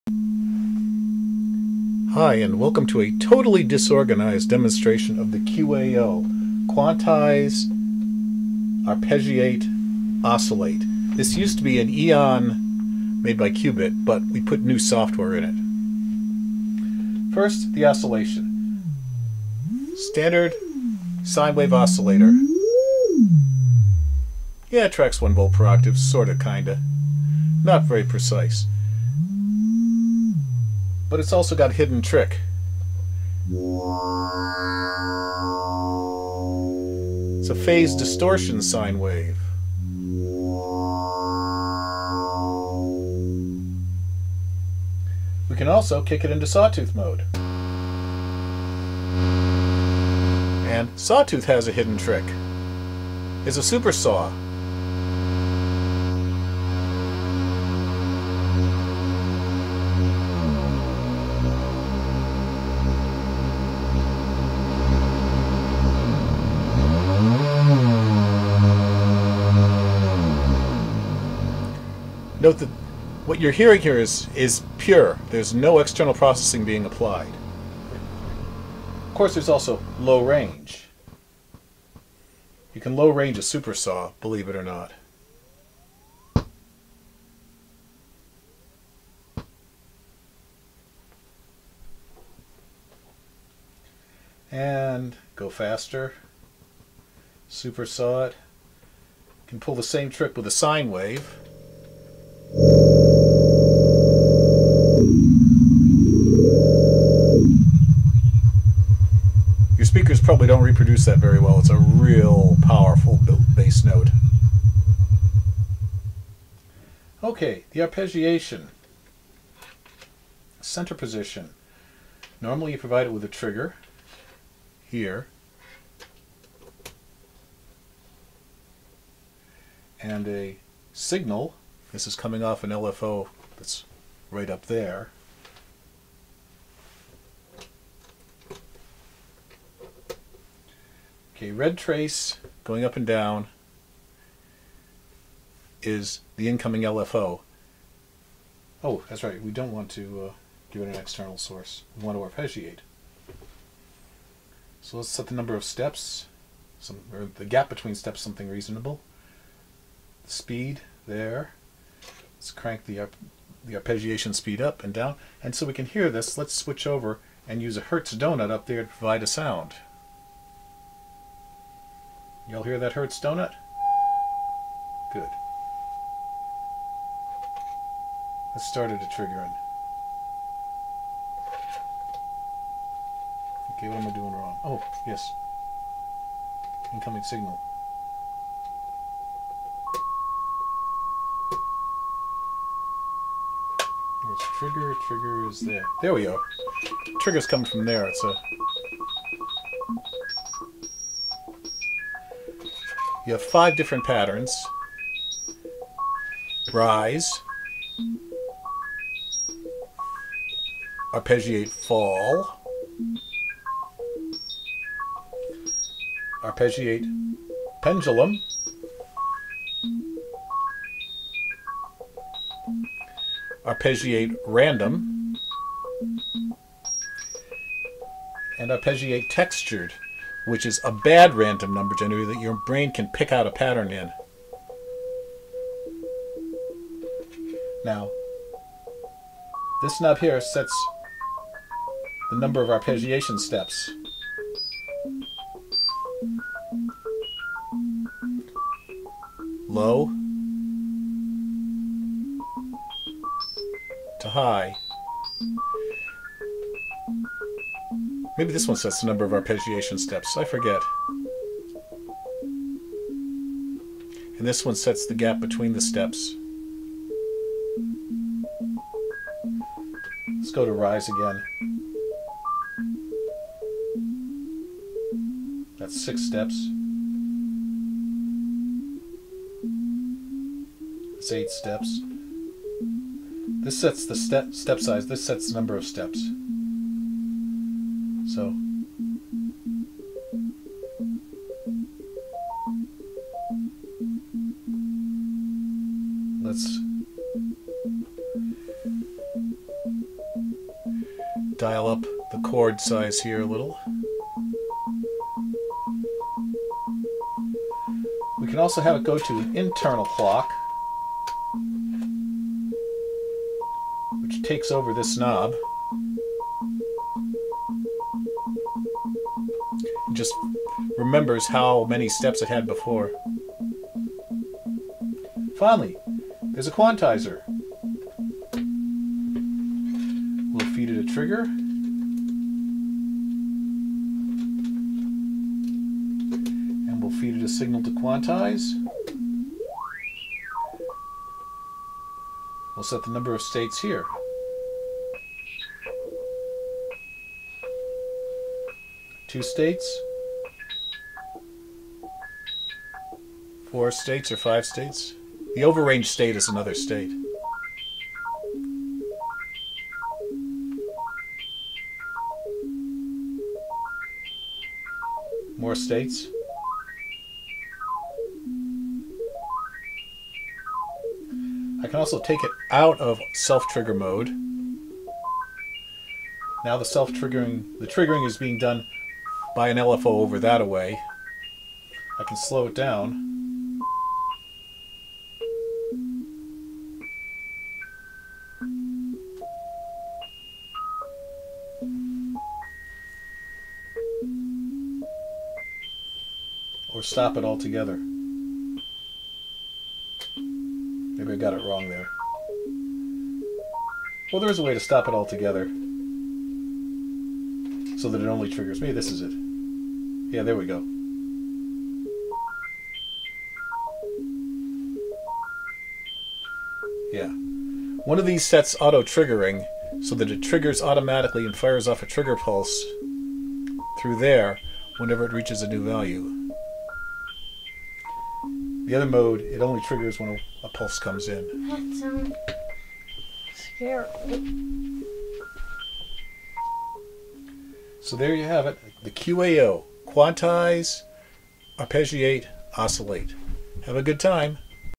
QAO - Alternate firmware for the Qu-bit EON module 20161128
The QAO (Quantize - Arpeggiate - Oscillate) is an alternative firmware for the Qu-Bit EON module.  I erased the processor as step 1, I did not disassemble nor reverse-engineer their software (therefore no copyright or DMCA violation). It was all microscope and voltmeter and scope and soldering iron work.

Anyway. onward to what the QAO does - Quantize, Arpeggiate, Oscillate.

The Quantizer has a variable number of buckets, an offsetting function, and can be triggered (and emit a trigger pulse) when ticked by external triggers, by an internal LFO, or by the CV input changing to a new bucket region.

The Arpeggiator has a variable number of steps to arppegiate, a variable span between each step, and can be triggered by an external trigger pulse or by an internal LFO.    It has five arpeggiation pattterns: up, down, pendulum, random, and "textured".  It emits a trigger pulse on each new output value.

The Oscillator has high (VCO) and low (LFO) range, hard sync, and two waveforms: a supersaw, and a TZPM phase distortion "supersine" (trigger is emitted when the waveform is above 0 volts).  1V/octave tracking isn't perfect; there are two pots I don't quite understand yet.

The firmware is GPLed, free and open source.

Further details and the full source code in a format directly usable in the Arduino IDE can be found on the MuffWiggler forum.

Go have fun!